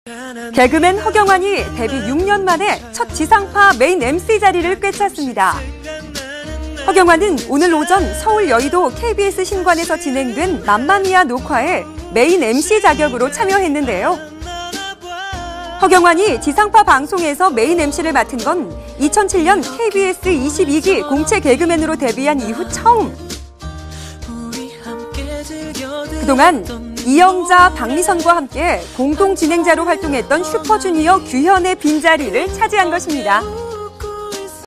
[Y-STAR] Heo Gyeonghwan entered the scene as a variety show host. (허경환, '맘마미아' 로 지상파 MC 데뷔)
개그맨 허경환이 데뷔 6년 만에 첫 지상파 메인 MC자리를 꿰찼습니다.

허경환은 오늘(24일) 오전 서울 여의도 KBS 신관에서 진행된 [맘마미아] 녹화에 메인MC 자격으로 참여했는데요.

허경환이 지상파 방송에서 메인MC를 맡은 건 2007년 KBS 22기 공채 개그맨으로 데뷔한 이후 처음.

그동안 이영자, 박미선과 함께 공동 진행자로 활동했던 슈퍼주니어 규현의 빈자리를 차지한 것입니다.